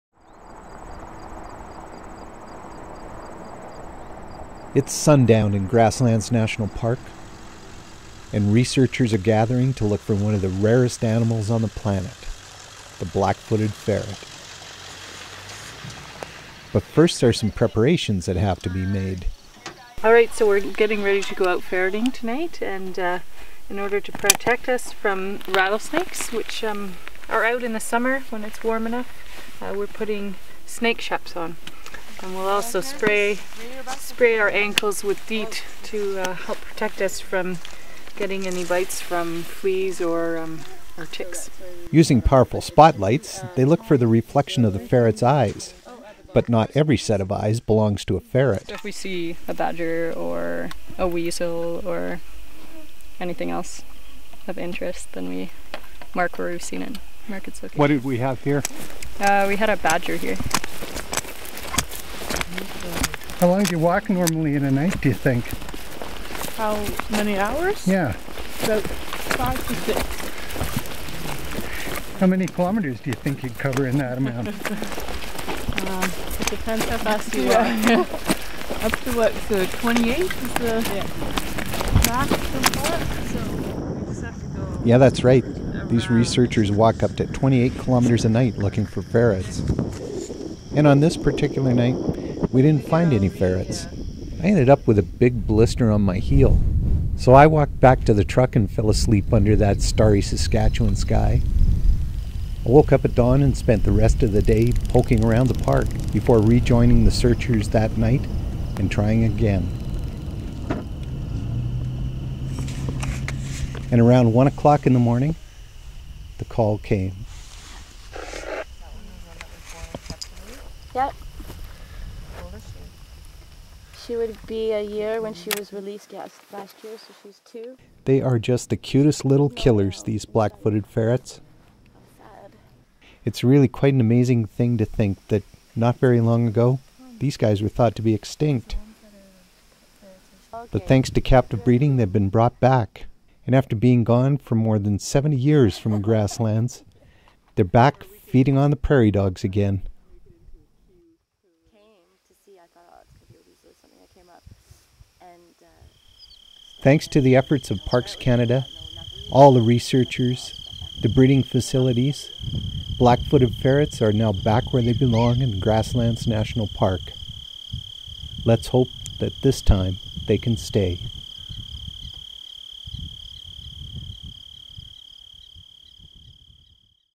Black-footed Ferrets
Just going through some of the videos I've put together over the years, I found this one I did in Saskatchewan's Grasslands National Park about the reintroduction of the very rare black-footed ferrets.